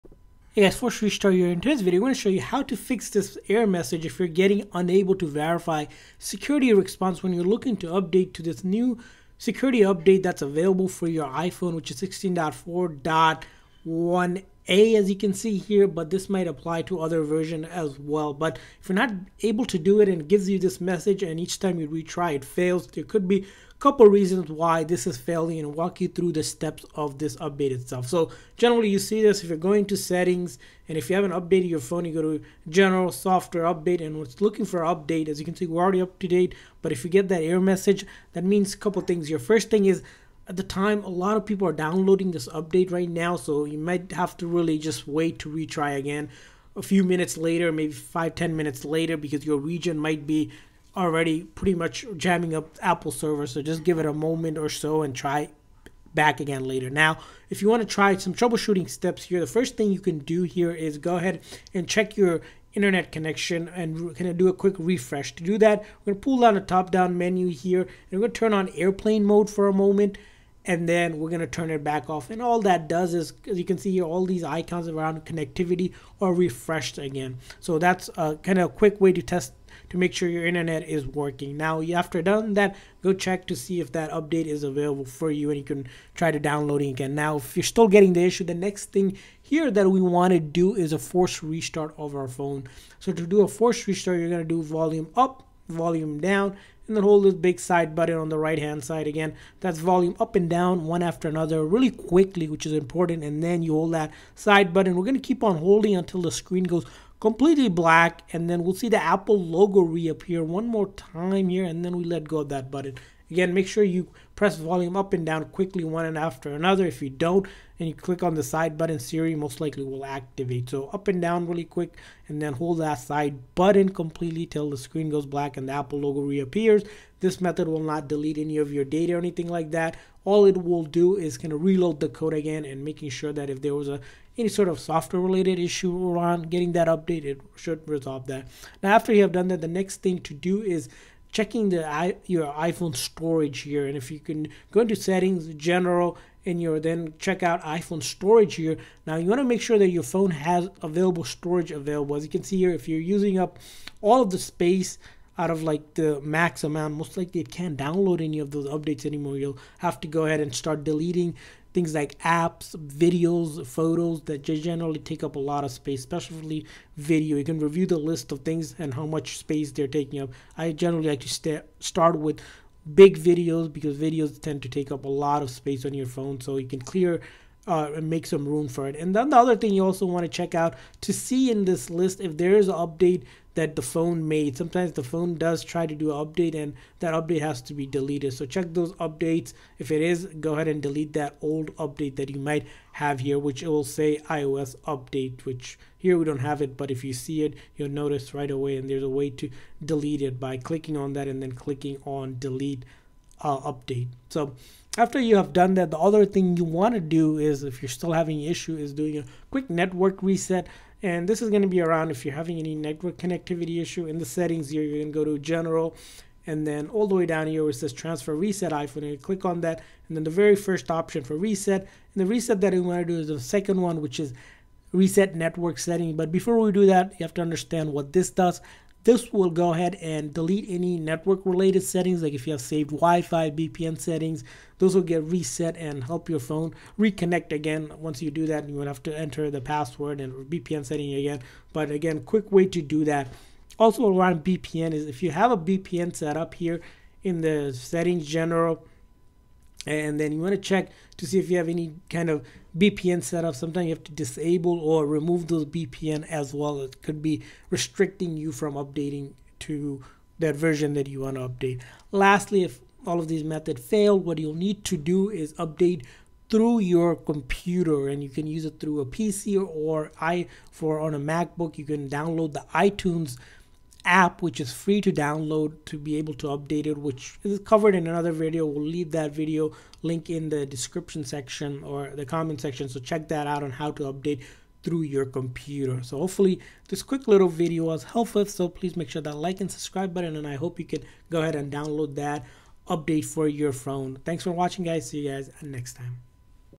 How to FIX Unable to Verify Security Response Error iOS 16.4.1 (a)
In this video we show you how to fix "Unable to Verify Security Response - iOS Security Response 16.4.1 (a) failed verification because you are no longer connected to the Internet.”